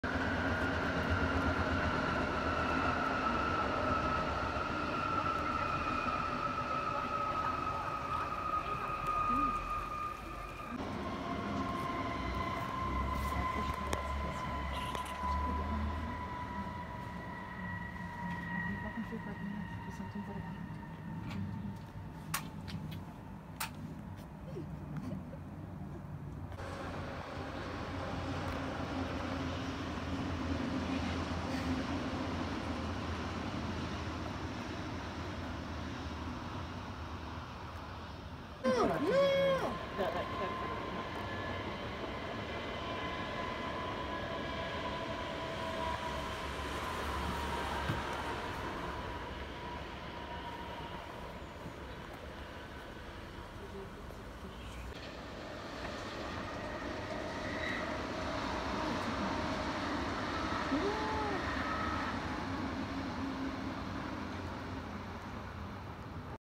Zugverkehr in Berkersheimer Weg
Zugverkehr an der Bushaltestelle Berkersheimer Weg mit Nahverkehrszügen.